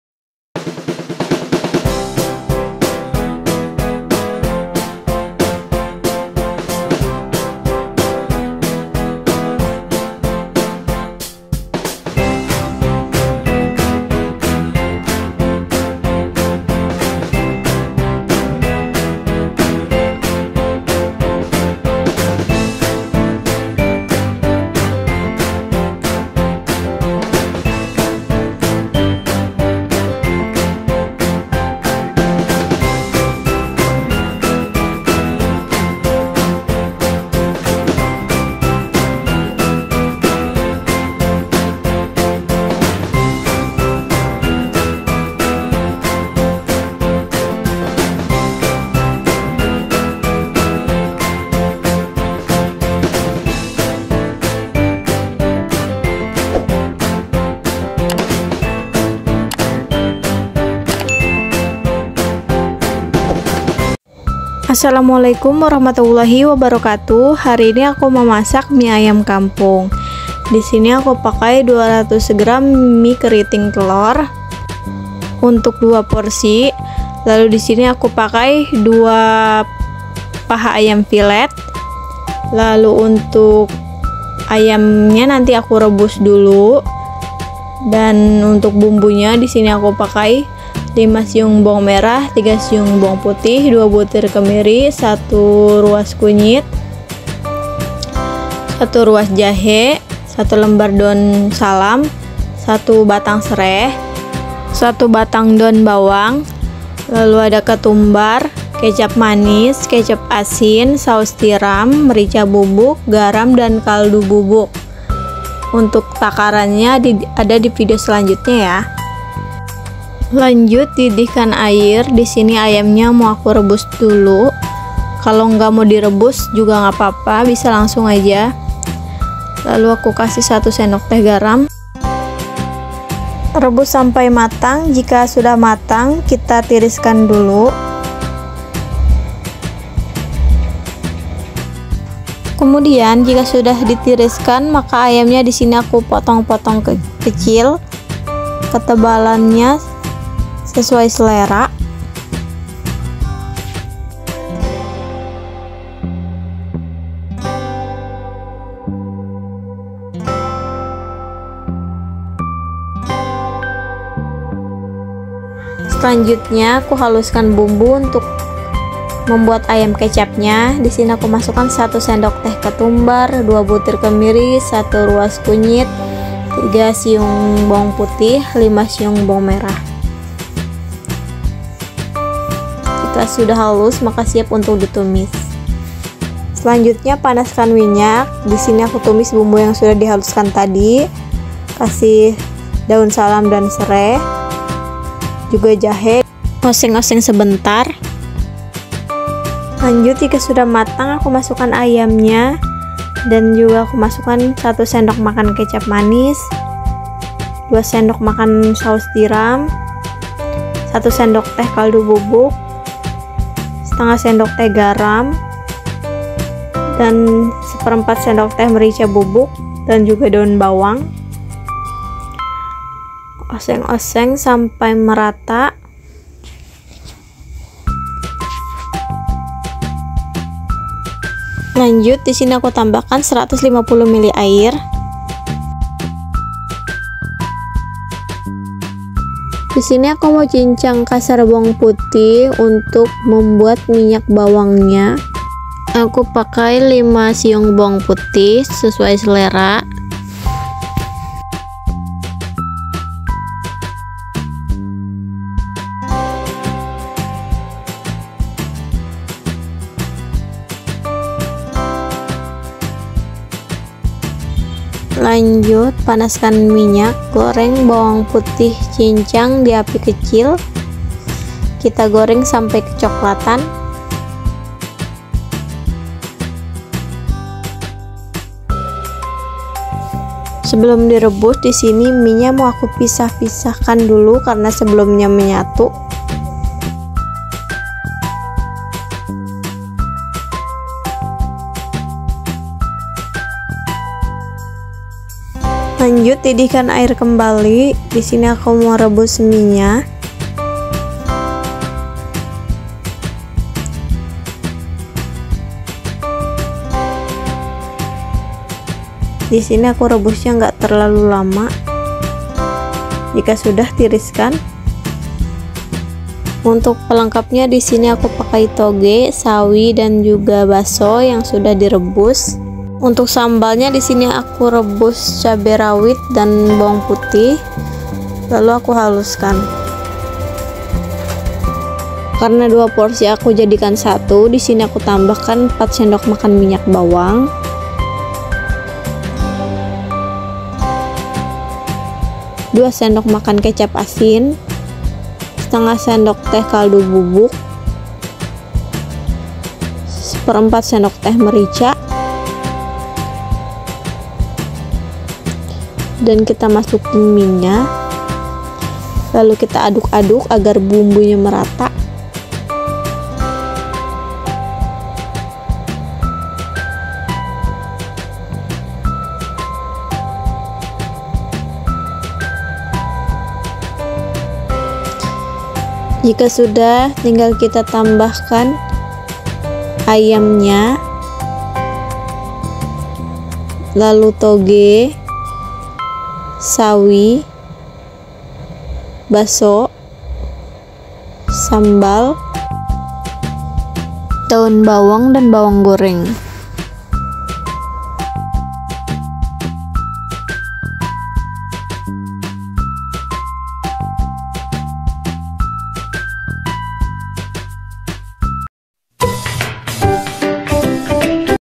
BIKIN MIE AYAM KAMPUNG ‼️ RASANYA ENAK BANGEET‼️
Resep Mie Ayam Kampung

Bahan:
200 gr mie telor keriting mentah
250 gr paha ayam fillet
1 batang daun bawang
1 lembar daun salam
1 batang sereh
1 ruas jahe
1 sdm kecap manis
2 sdm saus tiram
¼ sdt merica
1 sdt penyedap
½ sdt garam
150 ml air

Bumbu halus:
5 siung bawang merah
3 siung bawang putih
2 butir kemiri
1 sdt ketumbar
1 ruas kunyit

Minyak bawang:
5 siung bawang putih
50 ml minyak sayur

Sambal:
10 buah cabe rawit setan
3 siung bawang putih

Campuran di mangkok:
200 gr mie matang
4 sdm minyak bawang
2 sdm kecap asin
½ sdt penyedap
¼ sdt lada
air rebusan secukupnya

Pelengkap:
Sawi hijau
Toge
Sambal
bakso
saun bawang
bawang goreng

Langkah:
1. didihkan air, masukan paha fillet dan garam, rebus sampai matang lalu tiriskan
2. potong ayam dadu kecil
3. haluskan bawang, ketumbar, kunyit, kemiri
4. panaskan minyak, masukan bumbu halus, sereh, daun bawang, tumis sebentar
5. masukan kecap manis, saus tiram, garam, kaldu bubuk, lada, irisan daun bawang, air, masak hingga matang
6. cincang kasar bawang putih, goreng sebentar di minyak panas dengan api kecil, goreng hingga kecoklatan lalu angkat
7. didihkan air, rebus mie hingga matang, tiriskan sebentar
8. siapkan mangkok, masukan minyak bawang, kecap asin, lada, kaldu bubuk, mie, air rebusan secukupnya, aduk hingga merata
9. tambahkan ayam kecap, rebusan sawi, toge, bakso, sambal, daun bawang, bawang goreng, dan mia ayam siap di sajikan